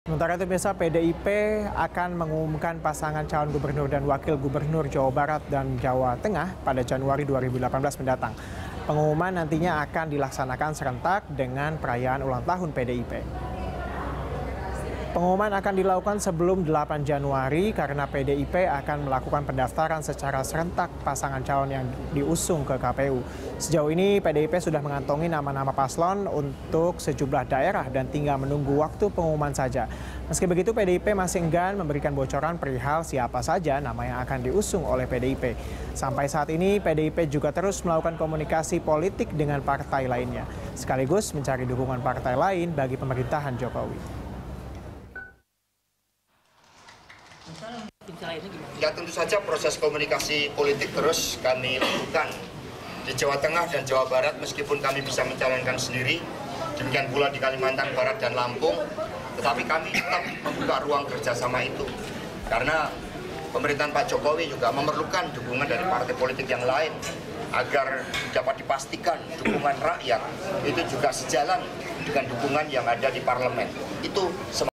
PDIP Umumkan Calon di Pilkada Jabar Januari 2018
PDIP akan mengumumkan pasangan calon gubernur dan wakil gubernur Jawa Barat dan Jawa Tengah pada Januari 2018. Pengumuman nantinya digelar serentak dengan perayaan ulang tahun PDIP.

Facebook.com/BeritaSatuTV
Youtube.com/BeritaSatu